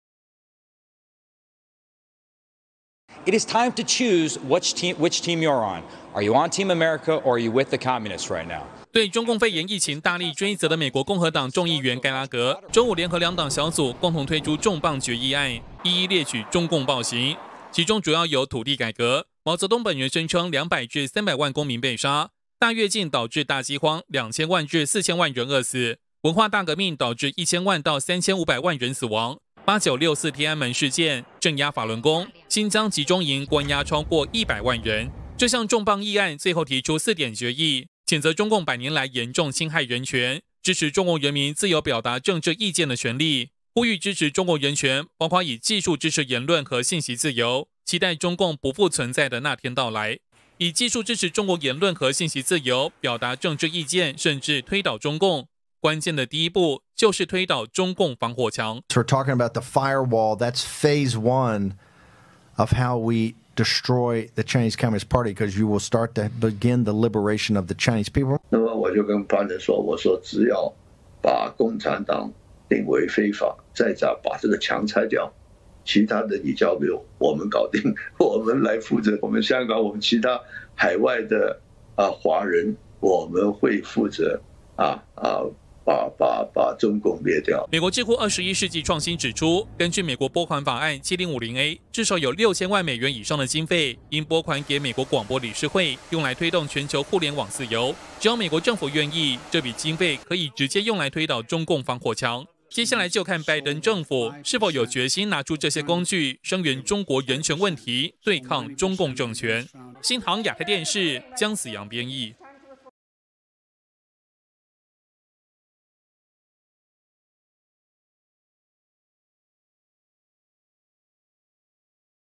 美國兩黨七一前重磅決議 譴責中共百年禍害人權｜@ChinaBreakingNewsTV ｜20210627
今年的七月一日就是中共建黨一百周年，美國聯邦眾議院兩黨小組共同推出重磅決議案，詳細列舉中共建黨以來種種暴行，譴責中共迫害人權殺害數千萬人。議案最後呼籲以技術支持中國言論和信息自由，並期待中共不復存在的那天到來。美國智庫與專家分析，要推倒中共，關鍵的第一步就是推倒中共防火牆。